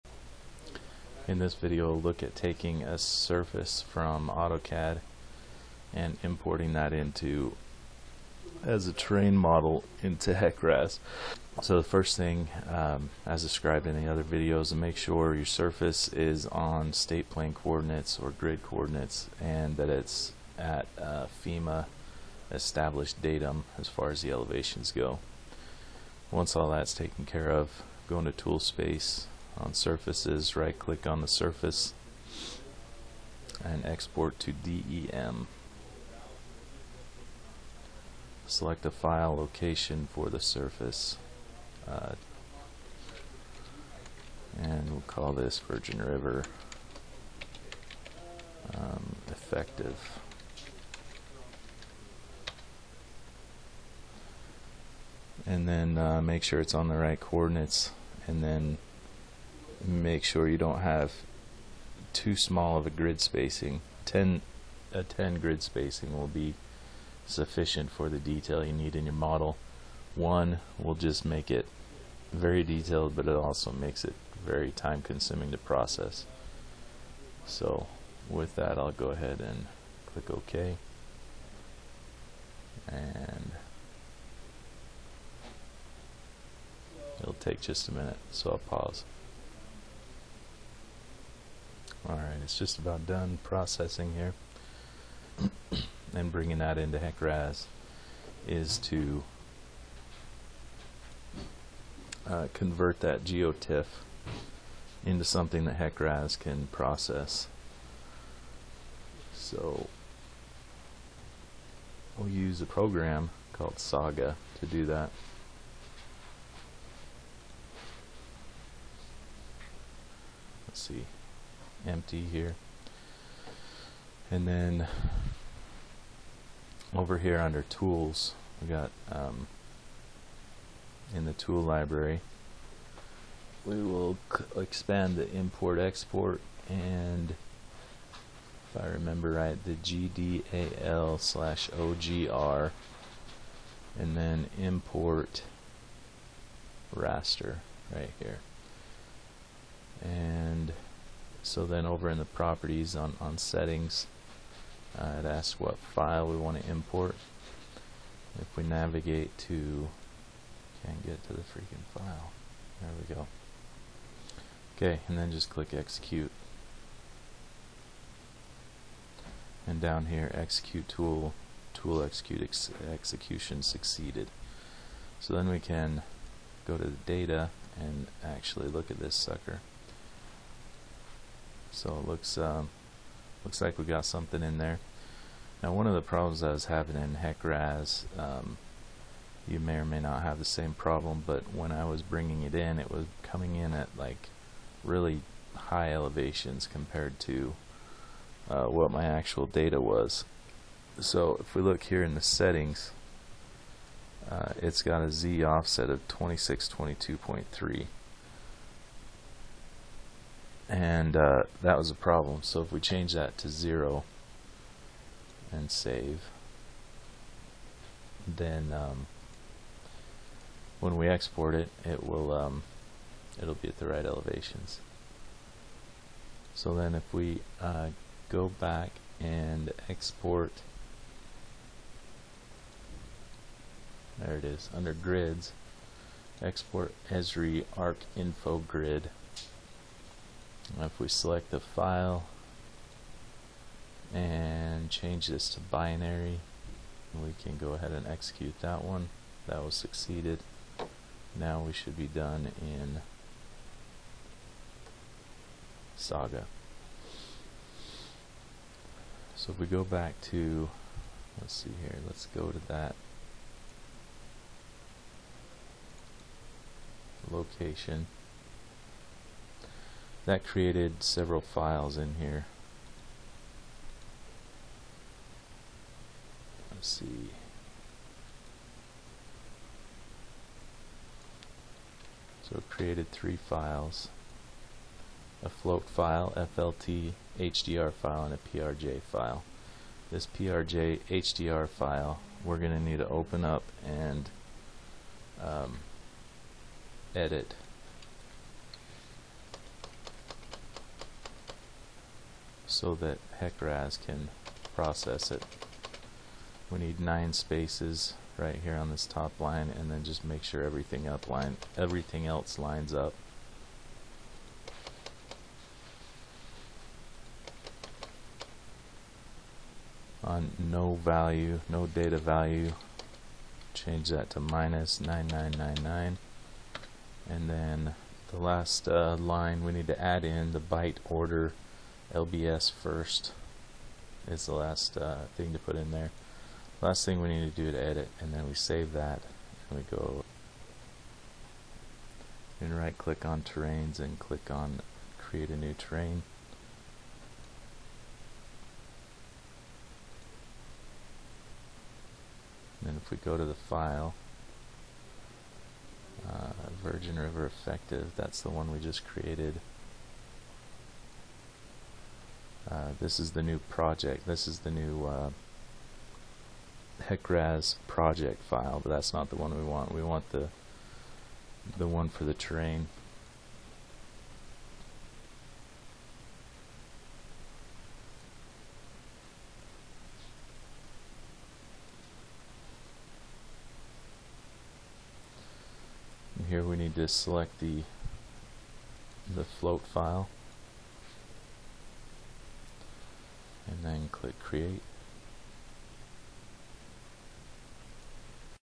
HEC-RAS Terrain From CAD
Use AutoCAD and SAGA to define a terrain model in HEC-RAS